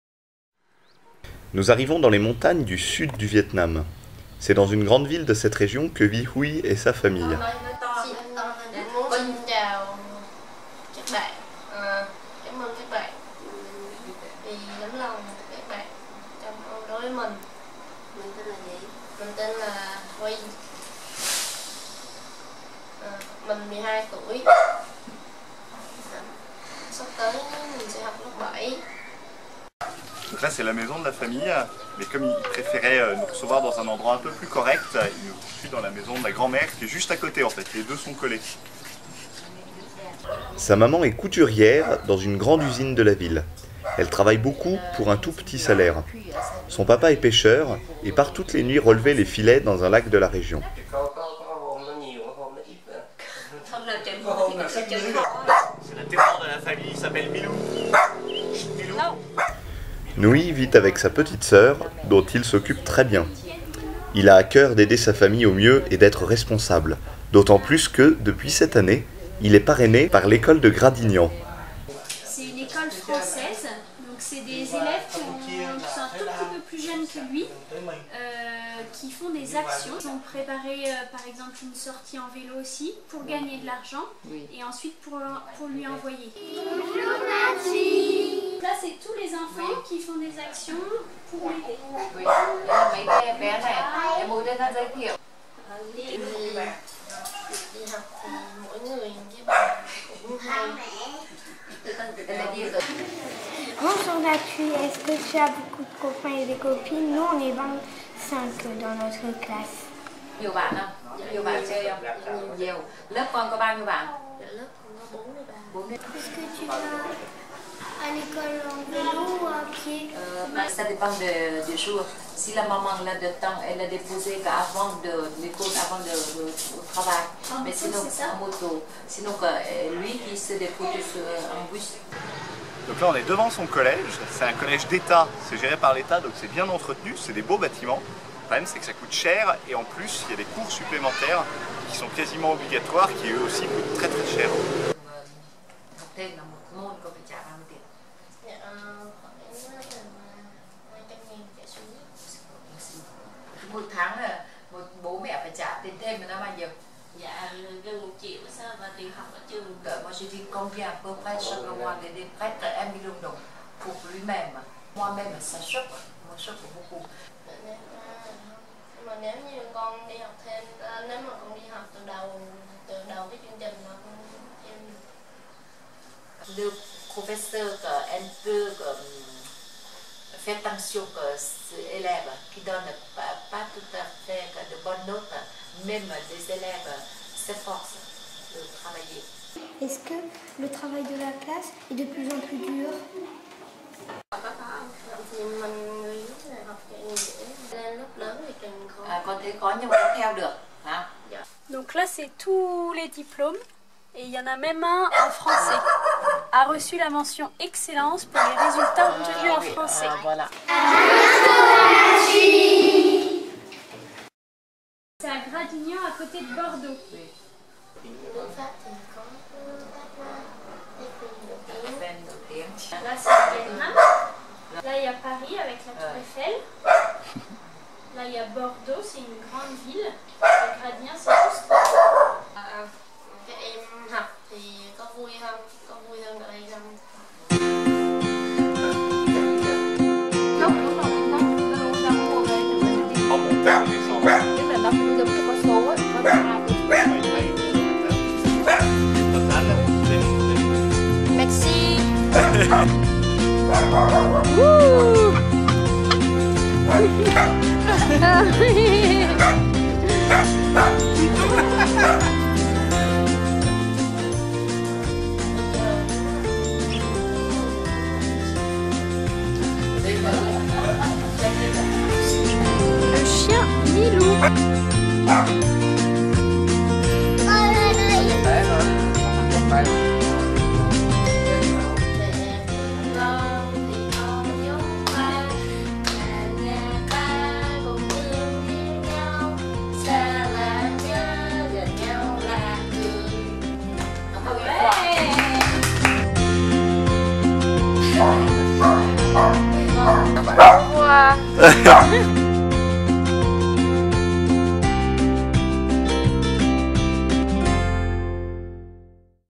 Huy : l'envie de réussir
Dans le centre du Vietnam, les cours supplémentaires payants sont obligatoires pour quiconque veut réussir sa scolarité. Huy et sa famille n'ont pas les moyens de payer 1 million de dongs par mois pour ces cours. Heureusement, Huy est parrainé par l'école de Gradignan (33), et peut continuer d'apprendre dans de bonnes conditions !